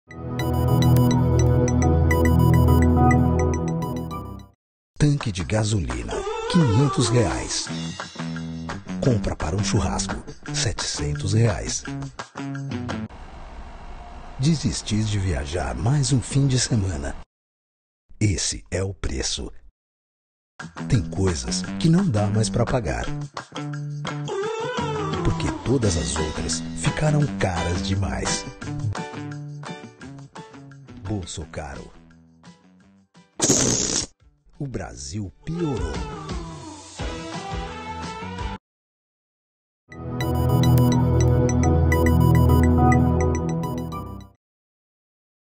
Movimento BolsoCaro critica alta no preço dos combustíveis, carne e passagens aéreas
Políticos e personalidades de oposição ao governo do presidente Jair Bolsonaro lançam movimento BolsoCaro e divulgam vídeos com críticas à situação econômica do Brasil e ao aumento da inflação.

As peças falam que certas despesas ficaram inacessíveis e culpam o presidente pela alta de preços, dizendo que "o Brasil piorou".

Os vídeos são uma paródia do logotipo e dos comerciais do Mastercard.

O assunto "O Brasil Piorou" ficou no 1º lugar dos Trending Topics do Twitter na 4ª feira (1º.jun.2022).

Entre as pessoas que participaram do movimento, estão o ator Gregório Duvivier, os deputados federais Alessandro Molon (PSB-RJ), Orlando Silva (PC do B-RJ) e Ivan Valente (Psol-SP), o deputado estadual e presidente da Assembleia Legislativa do Rio de Janeiro, André Ceciliano, e o perfis oficiais do PC do B e do Psol.

Não há informações sobre quem produziu as peças. O Poder360 entrou em contato com as assessorias dos políticos, mas não obteve respostas.